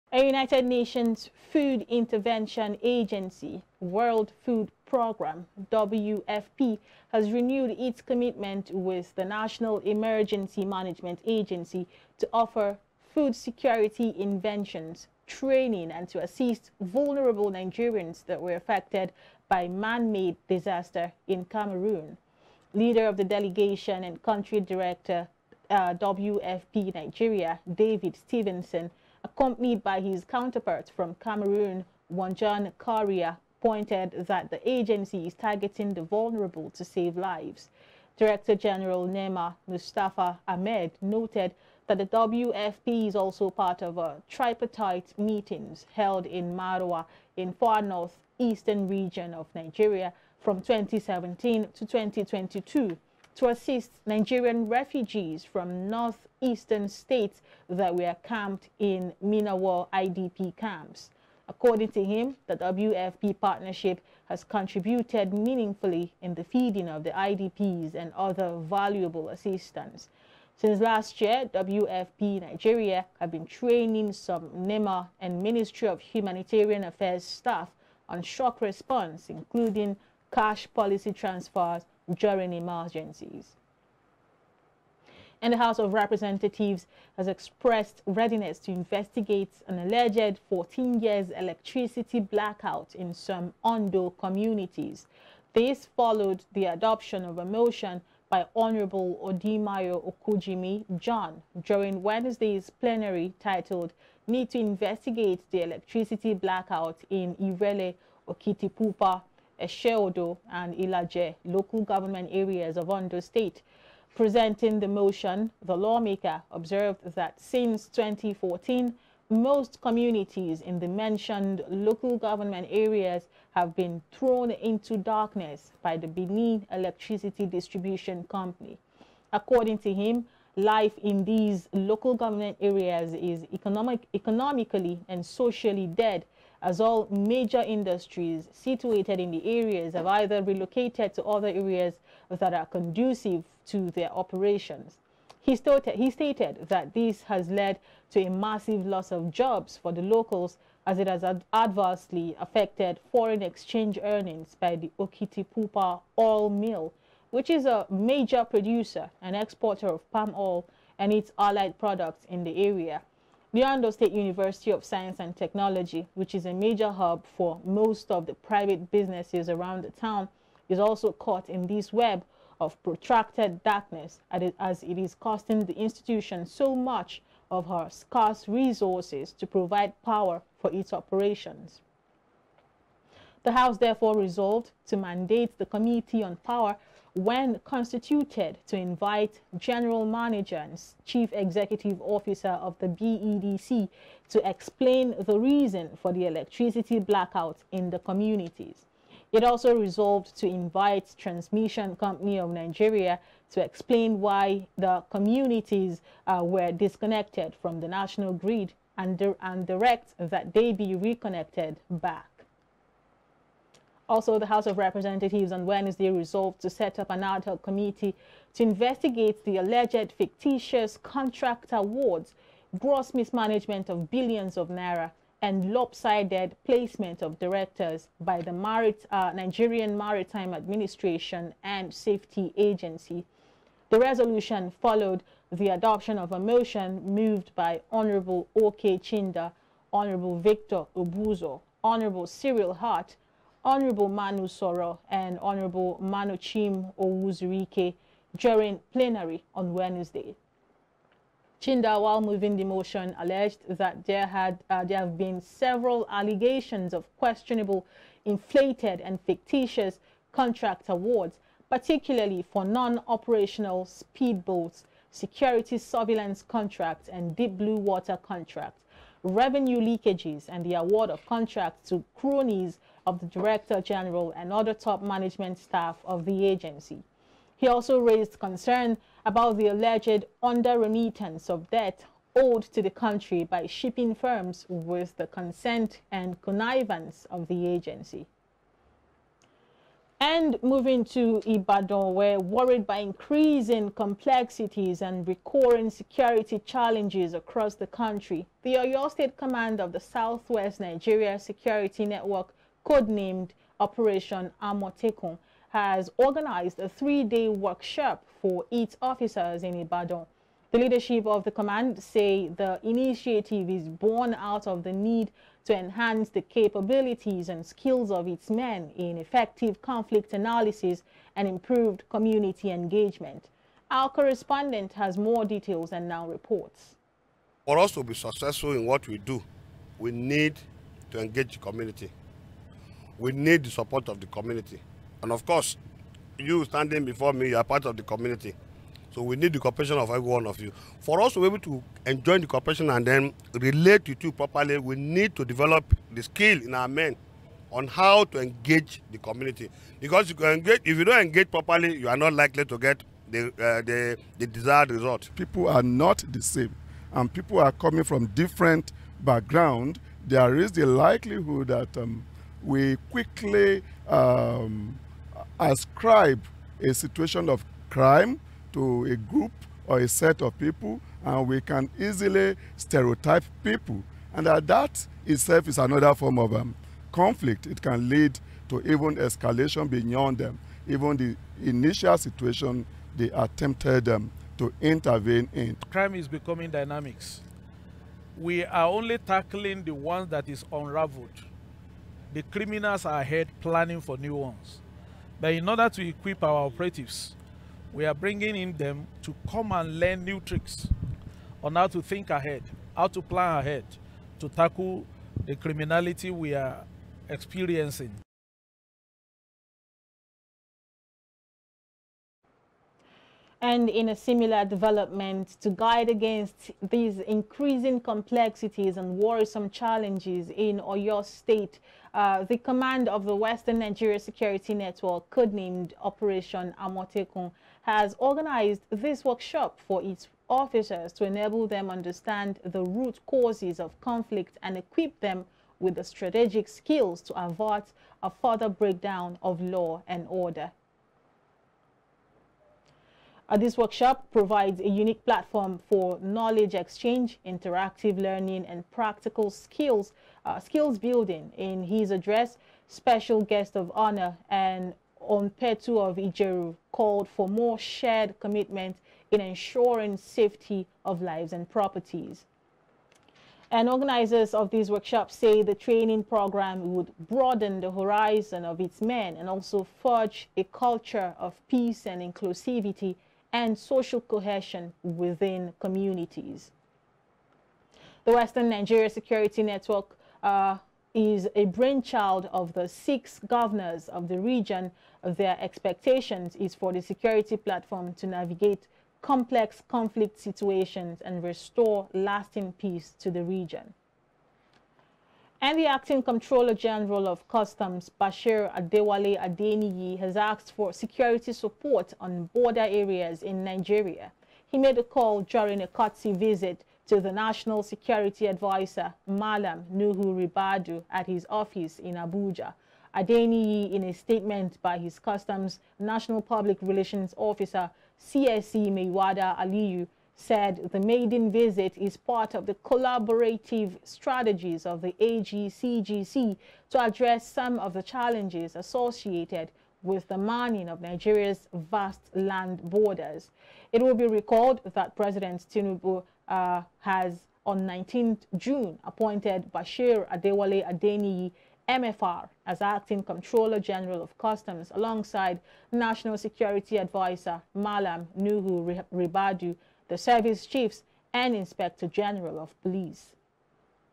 We Found A State That Has Suffered Electricity Blackout For 14 years
There is a state in Nigeria that has been on blackout for 15 years. Find out the state.